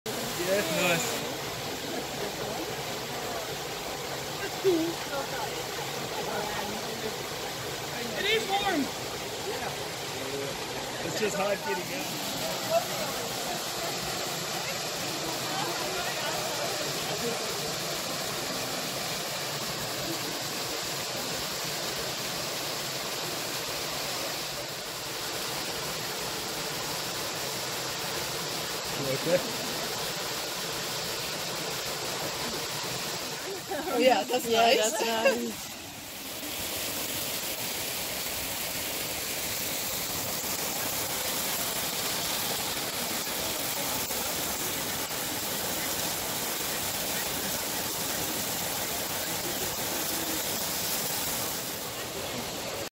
New Zealand Rotorua Wai-O-Tapu Waterfall Spout Bath (4K)
Video clips from my trip to Waterfall Spout Bath, a secret little place near the Lady Knox Geyser of Wai-O-Tapu near Rotorua, New Zealand